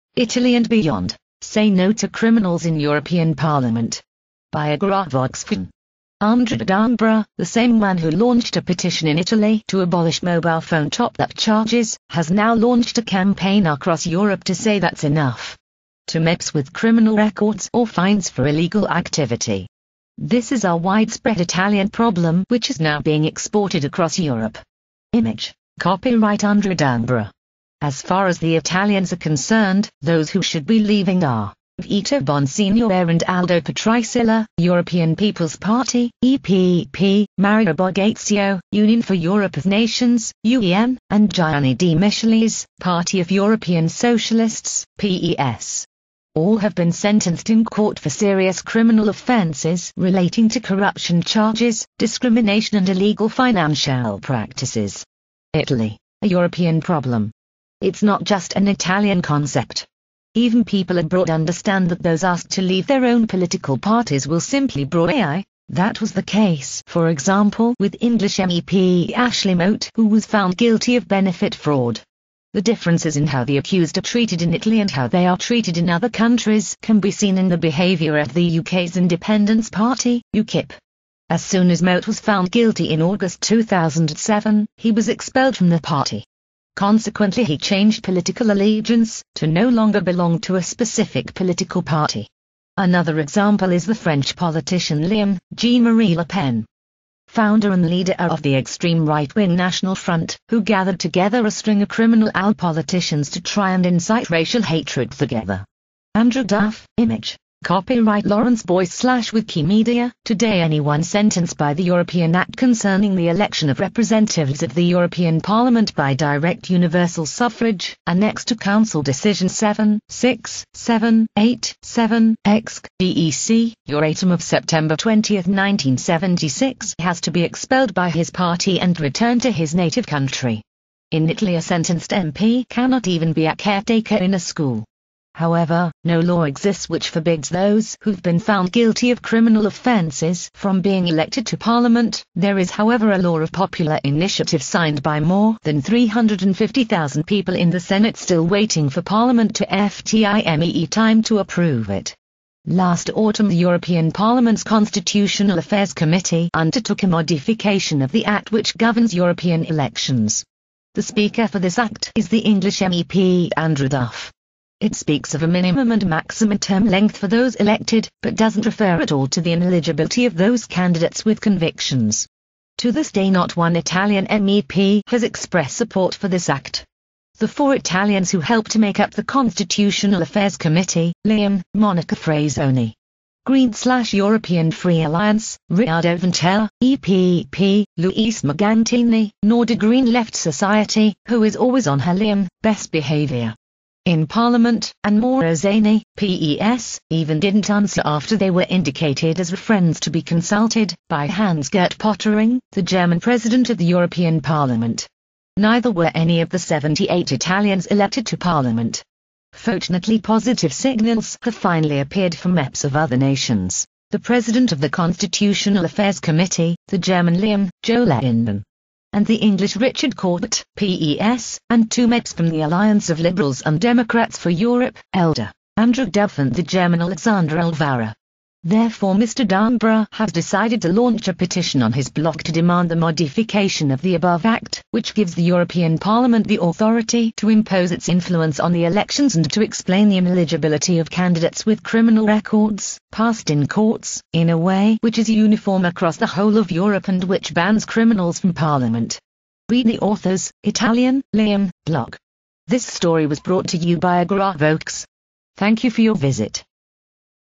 SAY NO TO CRIMINALS IN THE EUROPEAN PARLIAMENT
Andrea Dambra, the same man who launched a petition in Italy to abolish mobile phone top up charges, has now launched a campaign across Europe to say thats enough! to MEPs with criminal records or fines for illegal activity. This is a widespread Italian problem which is now being exported across Europe

As far as the Italians are concerned, those who should be leaving are: Vito Bonsignore and Aldo Patriciello (European peoples party, EPP), Mario Borghezio (union for Europe of nations, UEN) and Gianni de Michelis (party of European socialists, PES). All have been sentenced in court for serious criminal offences relating to corruption charges, discrimination and illegal financial practices.

Italy: a European problem

Its not just an Italian concept. Even people abroad understand that those asked to leave their own political parties will simply break away. That was the case for example with English MEP Ashley Mote who was found guilty of benefit fraud. The differences in how the accused are treated in Italy and how they are treated in other countries can be seen in the behaviour of the UKs independence party (UKIP). As soon as Mote was found guilty in August 2007, he was expelled from the party. Consequently he changed political allegiance, to no longer belong to a specific political party. Another example is the French politician Jean-Marie Le Pen, founder and leader of the extreme right-wing national front, who gathered together a string of criminal politicians to try and incite racial hatred together.

Today anyone sentenced by the 1976 European act which concerns the election of the European parliament representatives by direct universal suffrage has to be expelled by his party and return to his native country. In Italy a sentenced person cannot even be a caretaker in a school. However, no law exists which forbids those whove been found guilty of criminal offences from being elected to parliament (there is however a law of popular initiative signed by more than 350, 000 people in the senate still waiting for parliament to find the time to approve it). Last autumn the European parliaments constitutional affairs committee undertook a modification of the act which governs European elections. The speaker for this act is the English MEP Andrew Duff. It speaks of a minimum and maximum term length for those elected, but doesnt refer at all to the ineligibilty of those candidates with convictions.

To this day not one Italian MEP has expressed support for this act. The four Italians who help to make up the constitutional affairs committee - Monica Frassoni (green/ European free alliance), Riccardo Ventre (EPP), Luisa Morgantini (nordic green left society, who is always on her best behaviour in parliament) and Mauro Zani (PES), did not reply to my emails, sent on the advice of Hans-Gert Pottering, the German president of the European parliament. Neither were any of the 78 Italians elected to parliament. Fotunately positive signals have finally appeared from MEPs of other nations eg: the president of the constitutional affairs committee, the German Jo Leinen and the English Richard Corbett (PES), and two MEPs from the alliance of liberals and democrats for Europe (ALDE): Andrew Duff and the German Alexander Alvaro.

Therefore I have decided to launch an initiative on my blog to demand the modification of this act, which gives the European parliament the authority to impose its influence on the elections and to explain the ineligibility of candidates with criminal records (passed in courts) in a way which is uniform across the whole of Europe and which bans criminals from parliament.